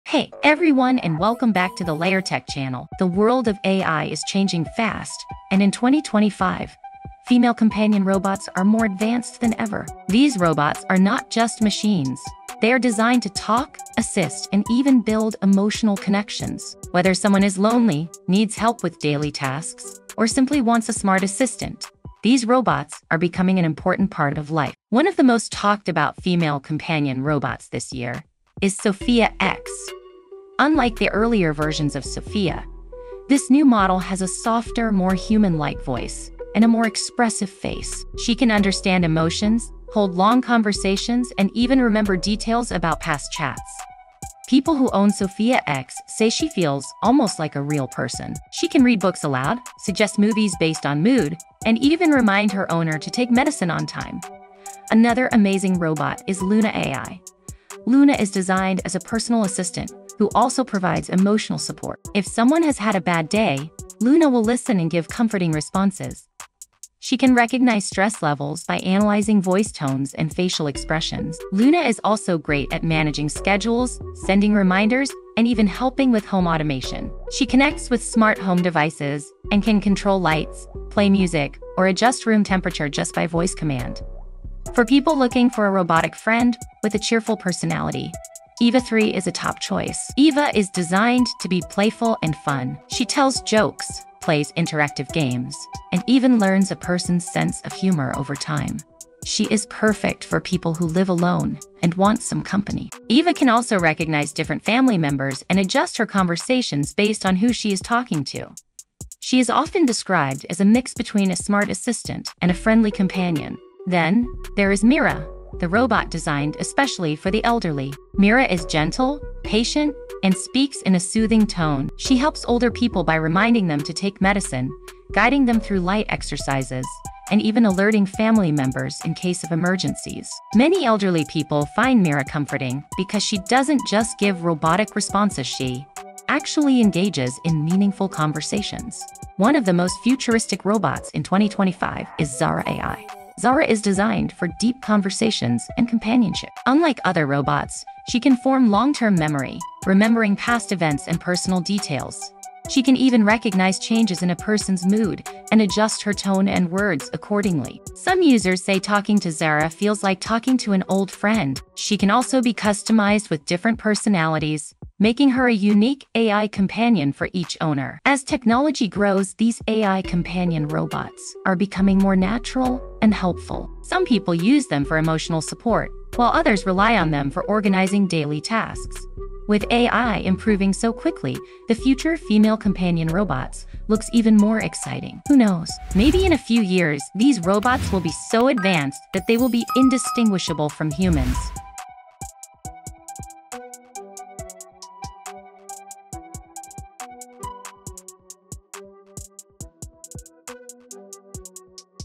You Won’t Believe What the Top Female Companion Robots of 2025 Can Do!
The Top Female Companion Robots of 2025 are more advanced than ever, pushing the boundaries of artificial intelligence and human interaction. These AI-powered assistants are designed to revolutionize companionship, offering cutting-edge features like emotional intelligence, realistic conversations, and adaptive learning. From helping with daily tasks to providing social interaction, these futuristic robots redefine what it means to have an AI assistant. But are they ready to replace human companionship? In this video, we explore the latest models, groundbreaking advancements, and the role of AI in shaping the future. Watch till the end to discover the most mind-blowing features of these next-gen robots.